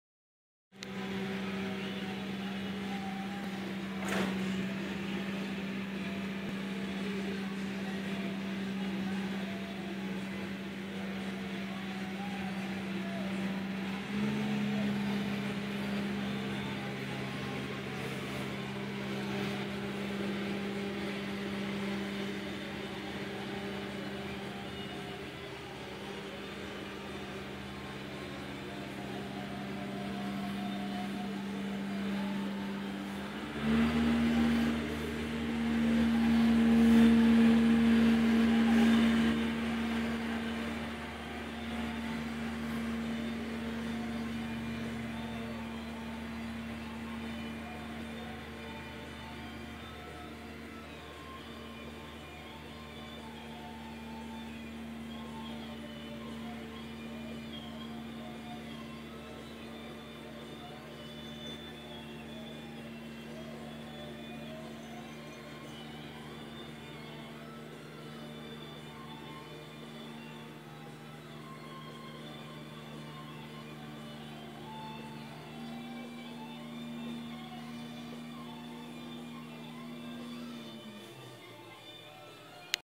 Harvest Dust Storm.avi
Dust Storm in front of house while Harvesting Walnuts